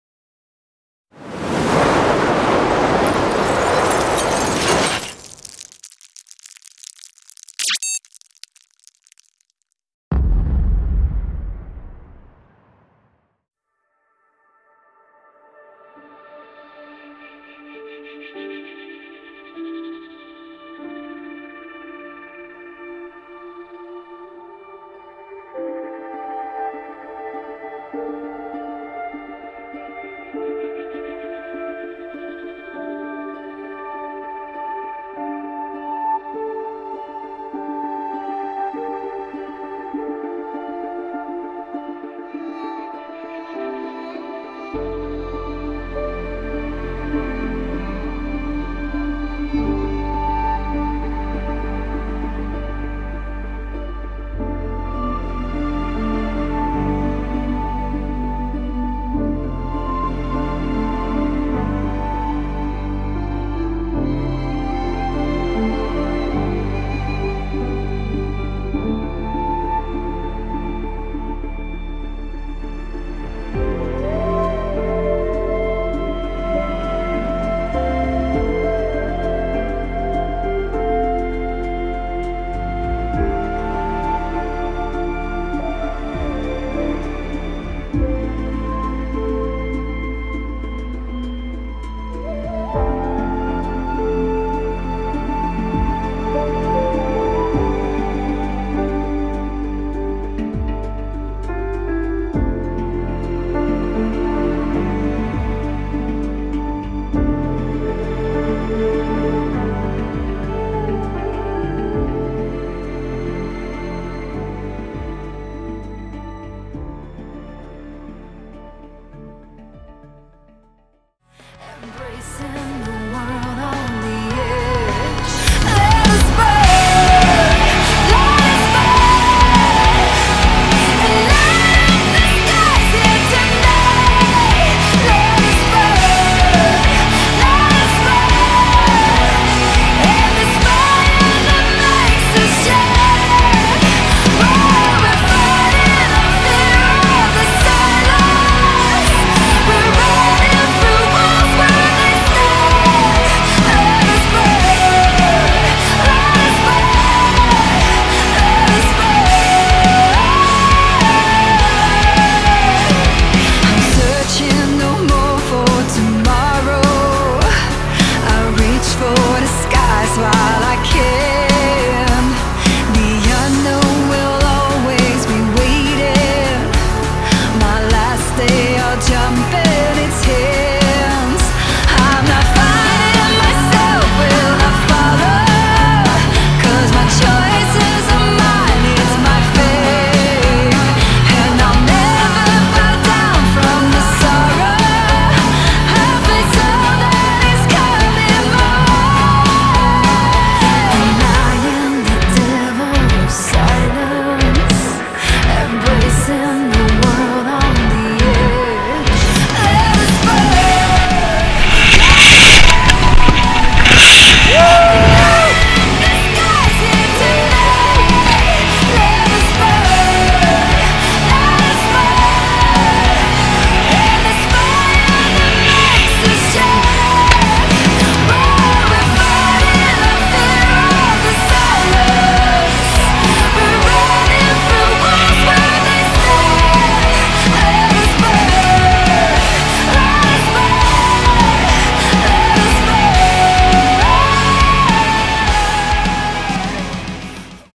Belarusian rope jumping
Weekend rope jumping in Minks, Belarus
Sound design, montage and rendering - Davinci Resolve 17